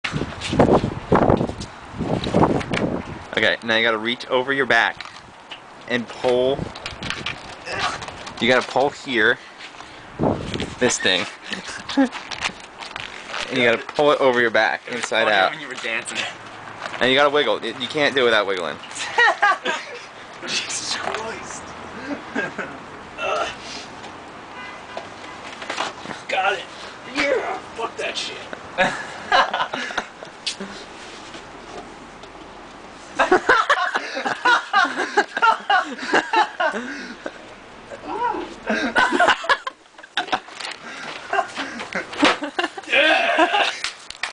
Brendan v.s. Rash Guard
my friend trying to take of a wet rash guard and looking like a doofus when it gets stuck on his head.